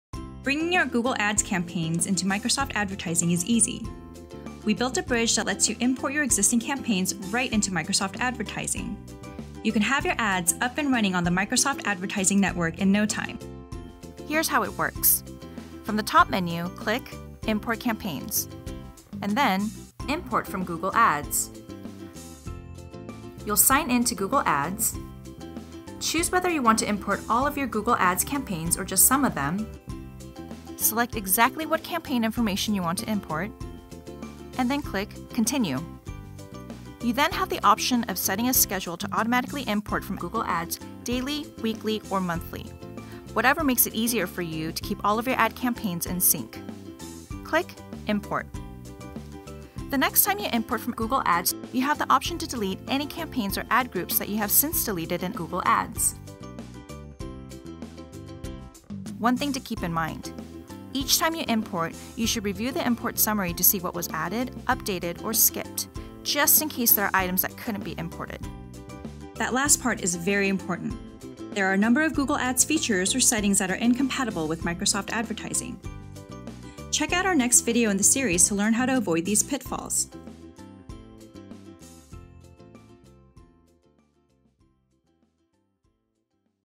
How to import campaigns from Google Ads into Microsoft Advertising
You can import your Google Ads campaigns directly into Microsoft Advertising. Then, set up automatic imports to keep your Google Ads and Microsoft Advertising campaigns in sync.